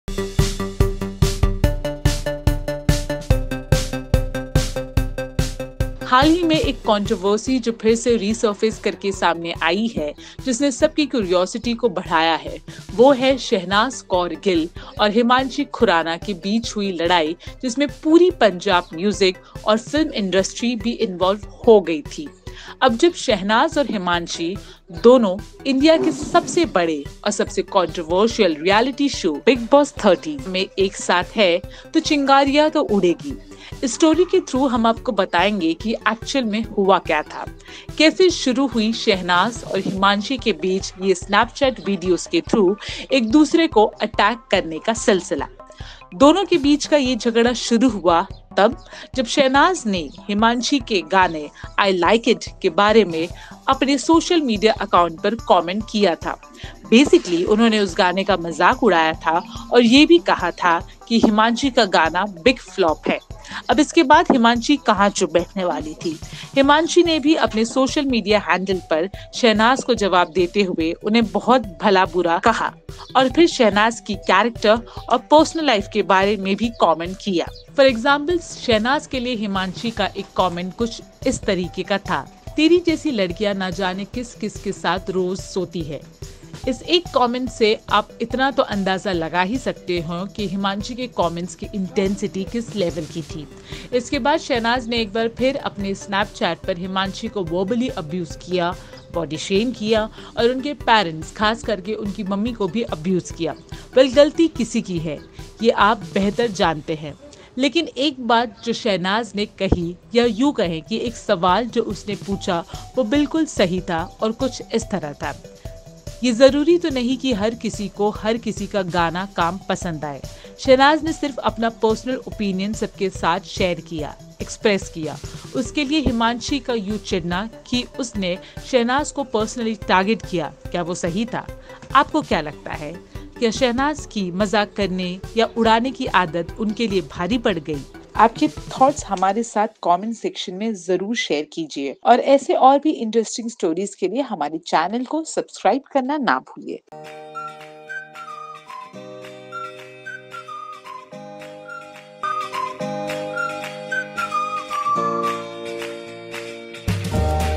Shehnaaz Gill VERSUS Himanshi Khurana | FULL STORY About Their Famous Bitter Controversy
ShehnazGill who is being loved by the audience for her current stint in BiggBoss13 has shared a very bitter past with HimanshiKhurana who recently entered the show a Wild Card.

Both Shehnaz and Himanshi have given bits and pieces of Information regarding their highly Controversial Fight. Through this story GlitzVisionUSA gives you an account of what had actually happened between the two leading ladies of the Punjab Music & Film Industry.